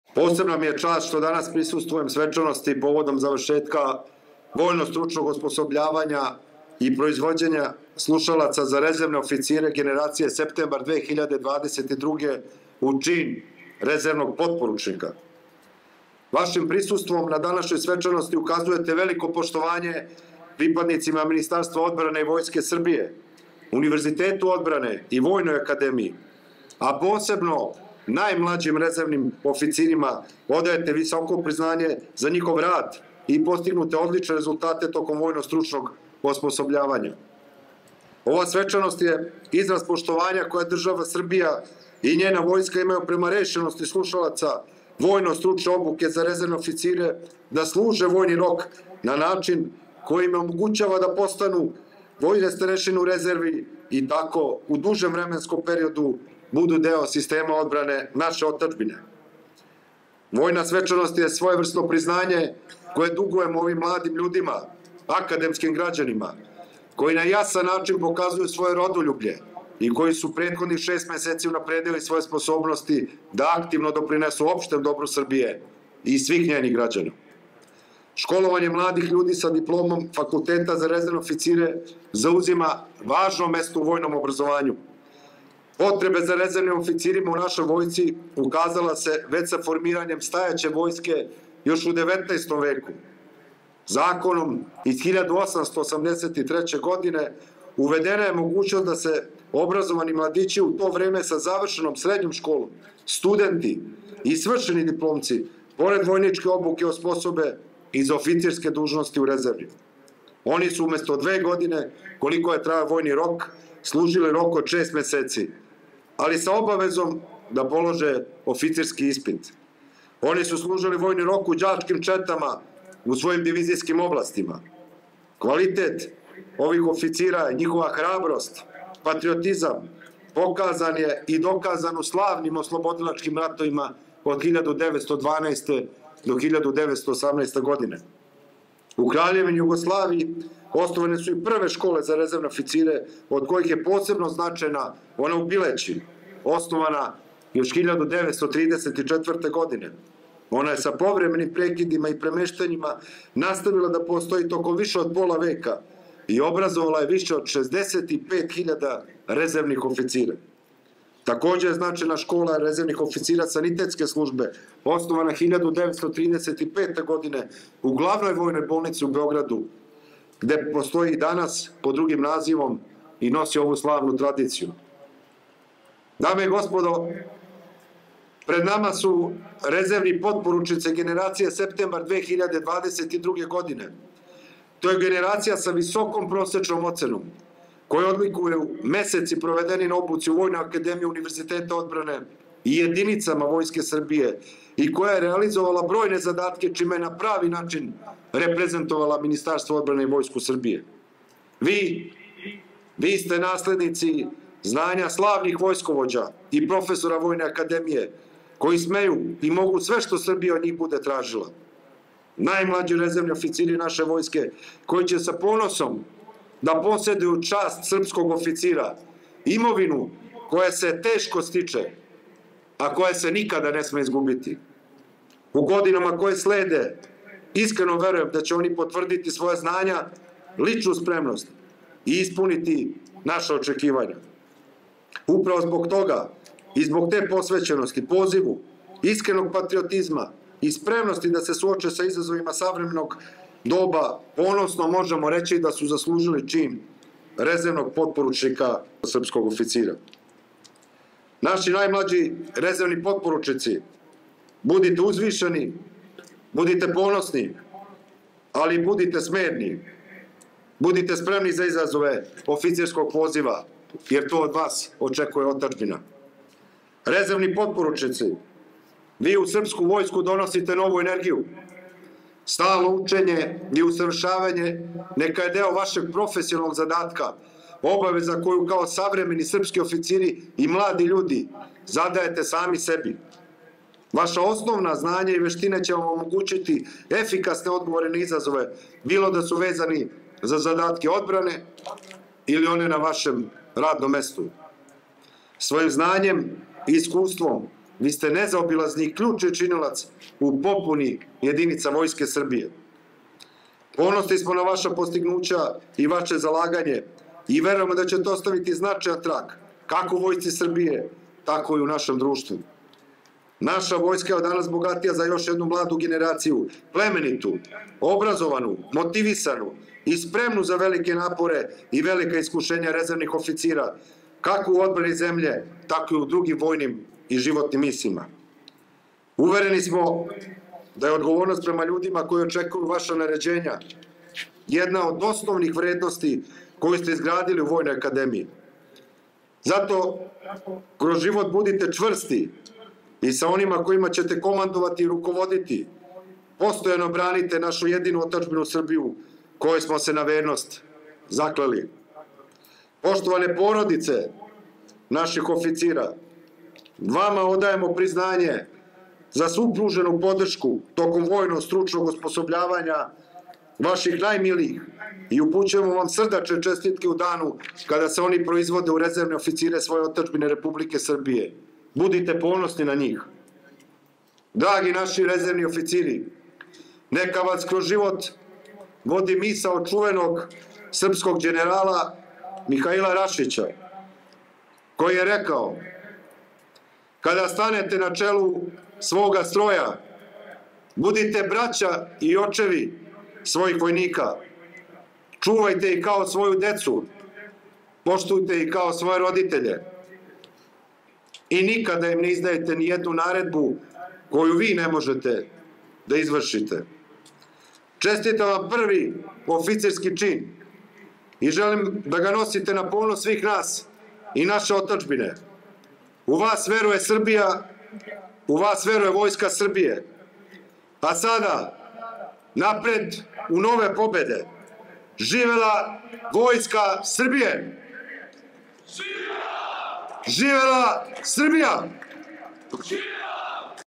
Obraćanje ministra Vučevića na svecanosti povodom završetka Kursa za rezervne oficire
Potpredsednik Vlade i ministar odbrane Miloš Vučević prisustvovao je danas svečanosti na Vojnoj akademiji povodom završetka vojnostručnog osposobljavanja slušalaca Kursa za rezervne oficire generacije „Septembar 2022”. Svečanosti su prisustvovali zamenik načelnika Generalštaba Vojske Srbije general-potpukovnik Petar Cvetković i prorektor Univerziteta odbrane pukovnik prof. dr Boban Đorović.

Na početku svečanosti, pročitan je Ukaz predsednika Republike i vrhovnog komandanta Vojske Srbije Aleksandra Vučića kojim je 84 slušaoca Kursa za rezervne oficire promovisano u čin rezervnog potporučnika.

Prema rečima ministra Vučevića, danas je pred nama generacija naslednika znanja slavnih vojskovođa i profesora Vojne akademije, koji smeju i mogu sve što Srbija od njih bude tražila.